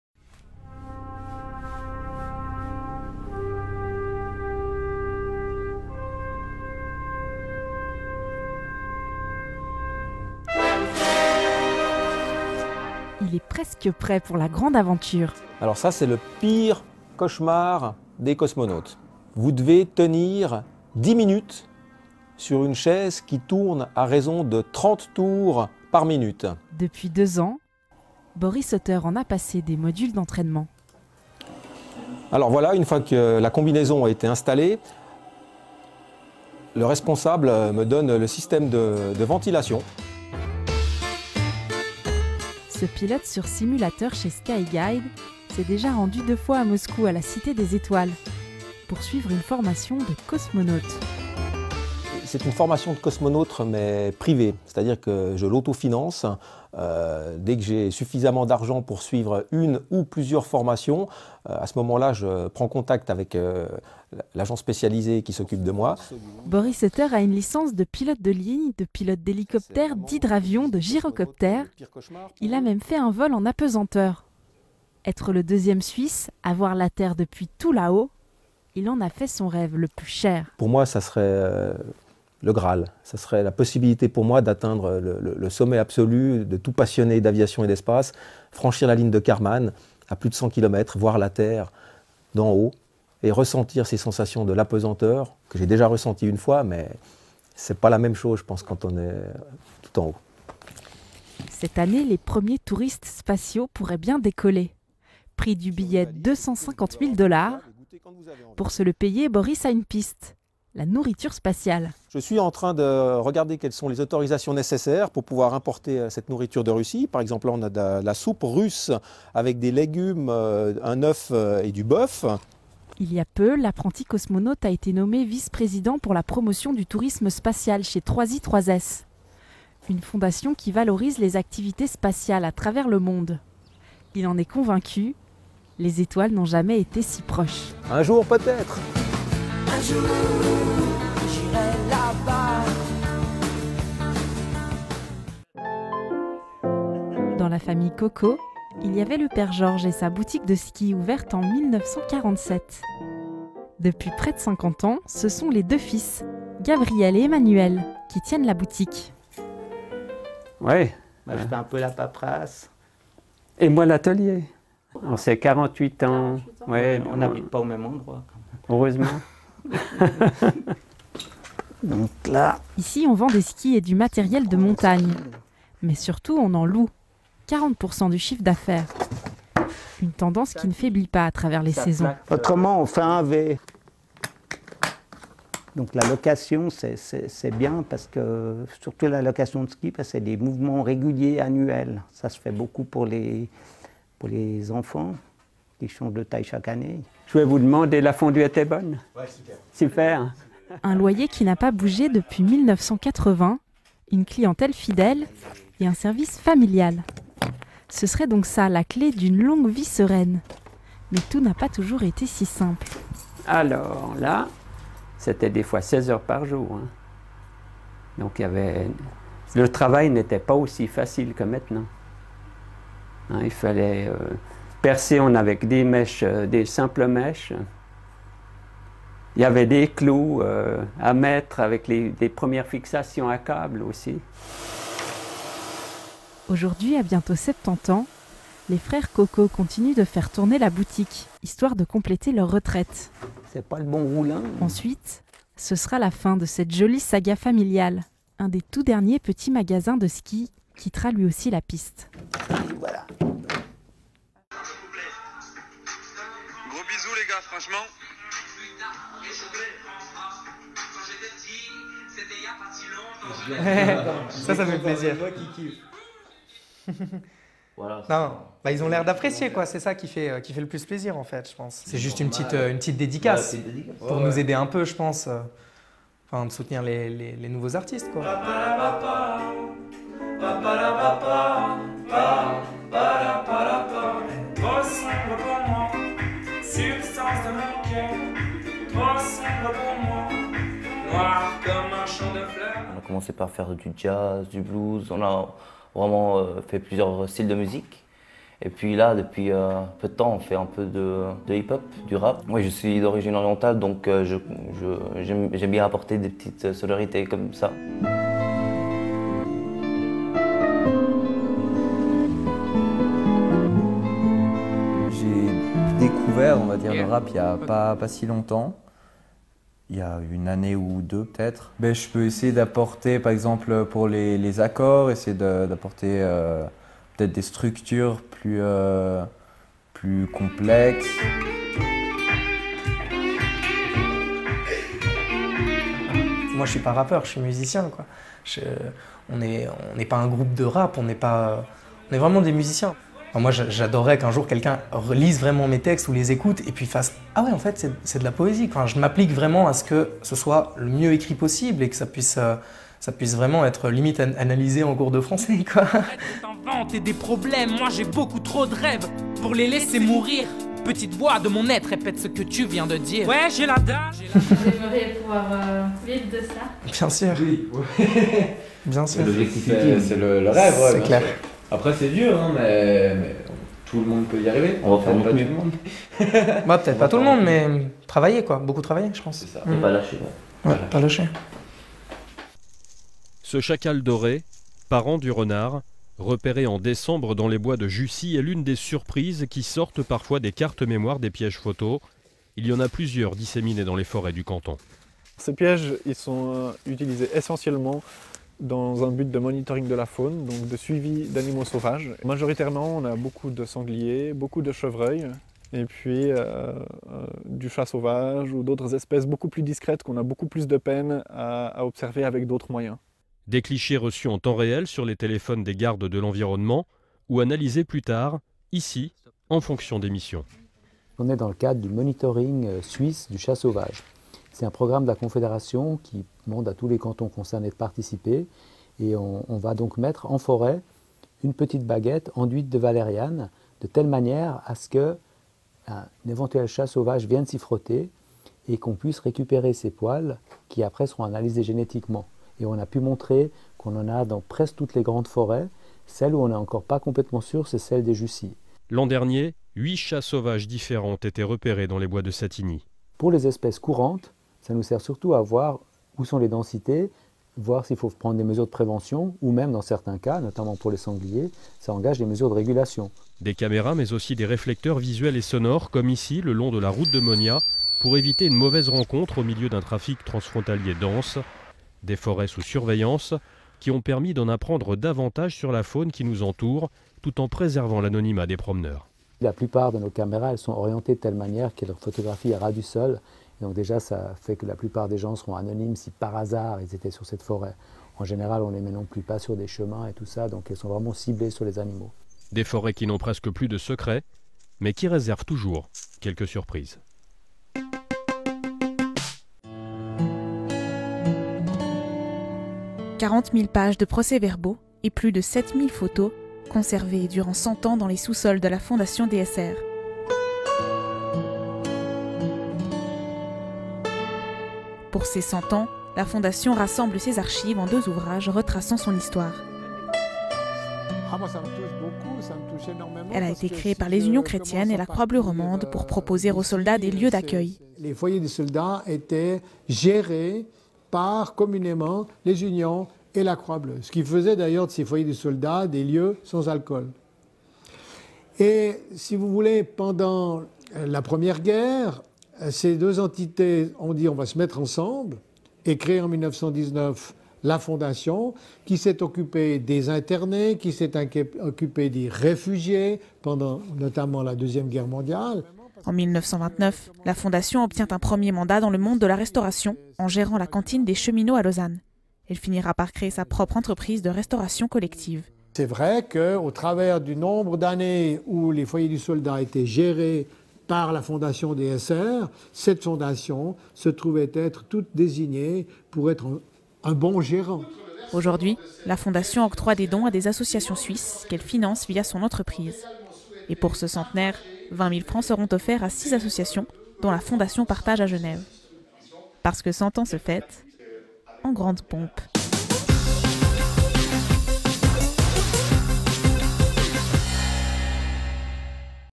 COSMONAUT TRAINING WITH BORIS OTTER, LEMAN BLEU TV
Reportage TV on Léman Bleu with Boris Otter, who aims to be the 2nd Swiss to fly to Space. February 2019.